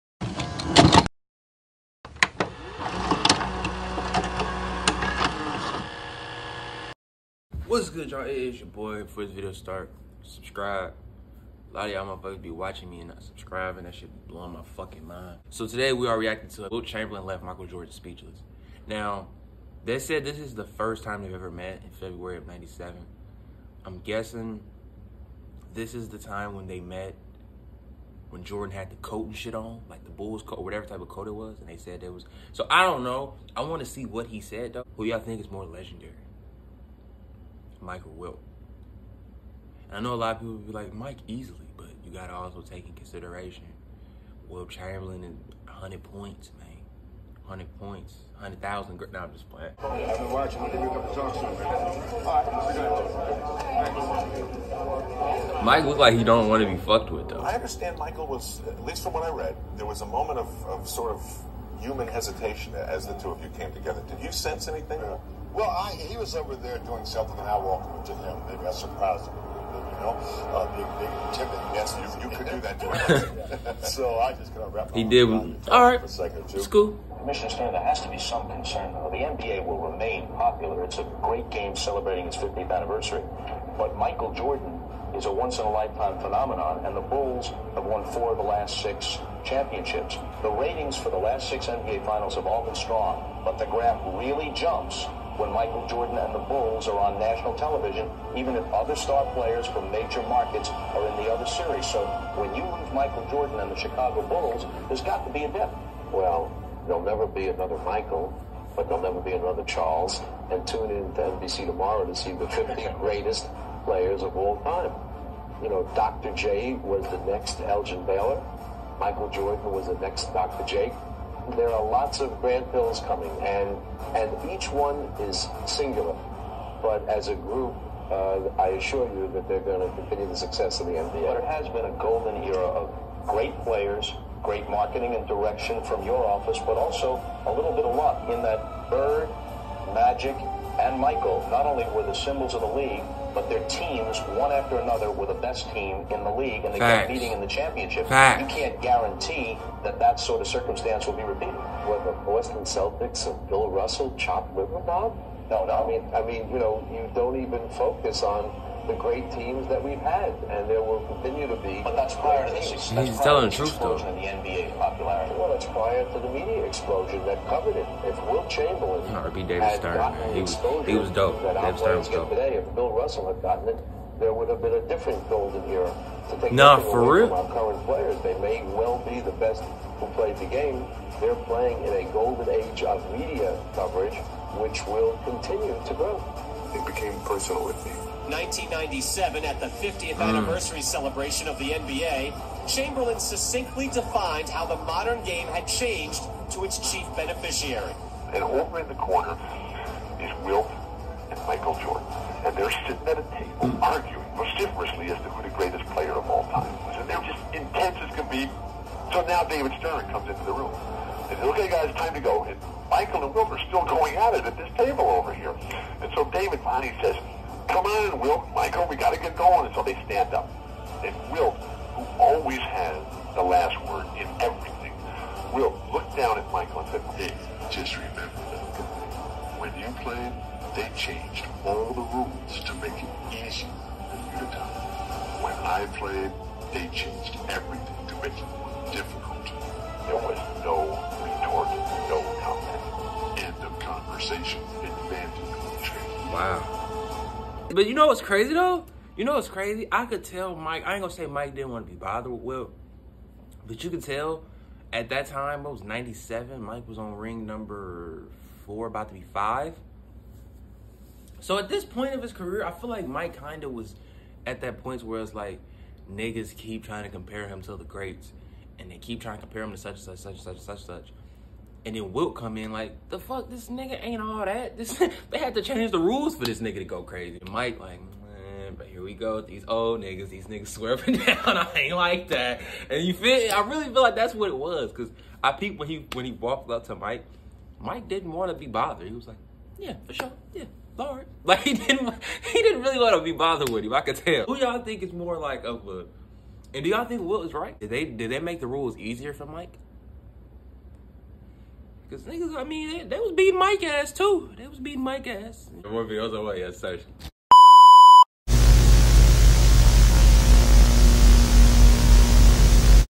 HE HUMBLED MJ! WHEN WILT CHAMBERLAIN LEFT MICHAEL JORDAN SPEECHLESS
I mean was he lying?.. but i can tell it was getting on MJ nerves. Y’all got to remember @ some point people were comparing MJ to alot of players before him like they’re doing NBA players now. Mike like mane 5 rings 😂.. More videos otw !